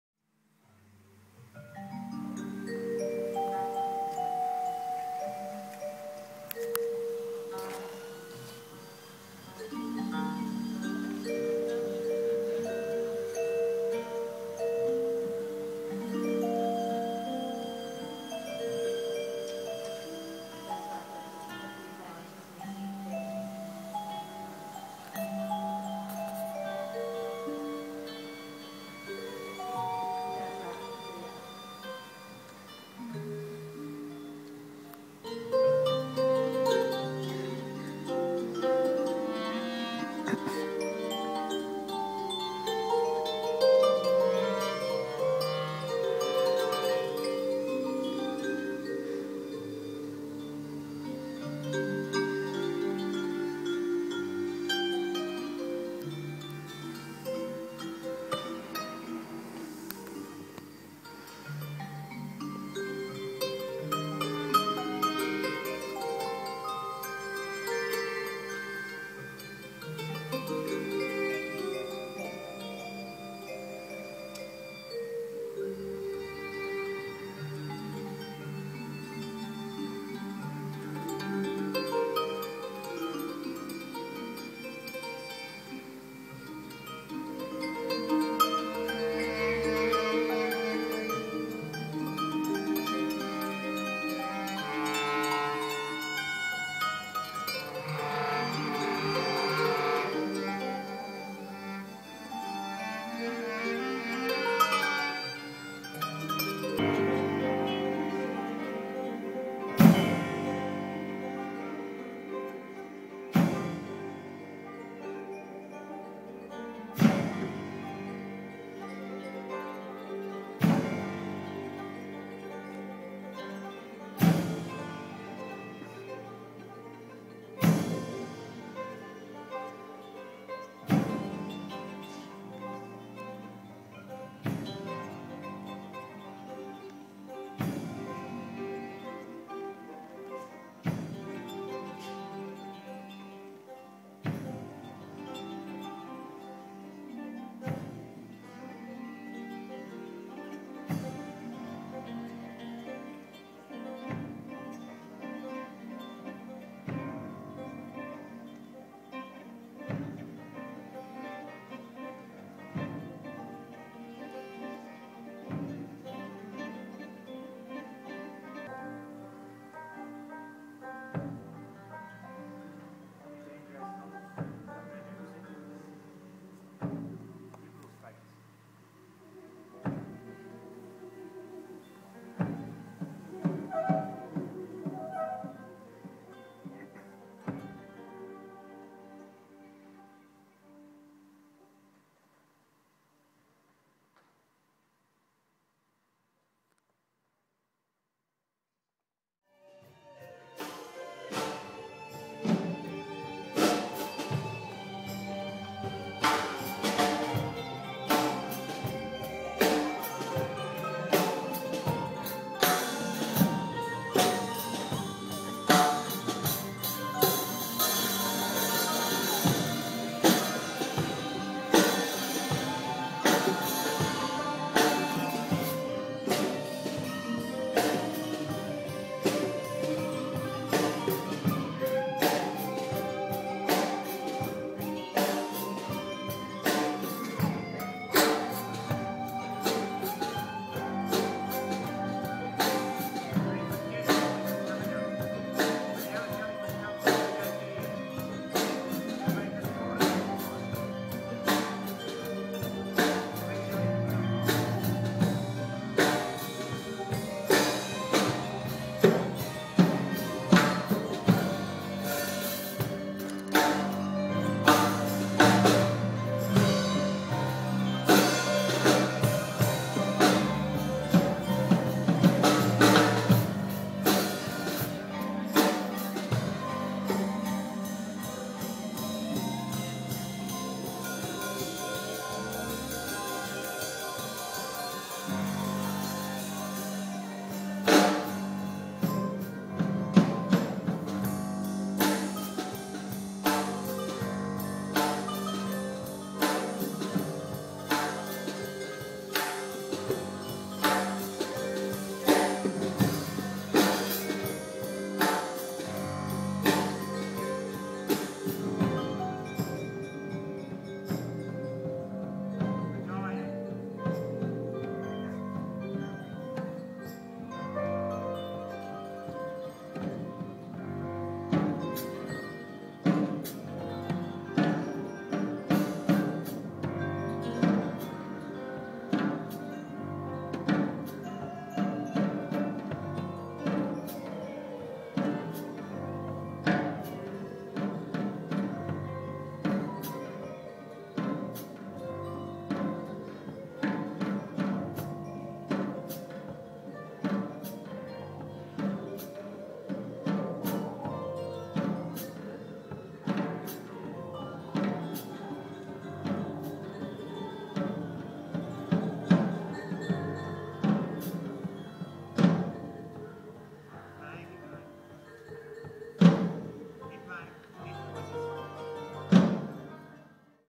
Cabaret Jellyfish
part of a Alice Springs Cabaret 2010 rehearsal.
Piece directed by Katelnd Griffin, part of  the Cabaret show.
Published in behalf of Katelnd Griffin at her request.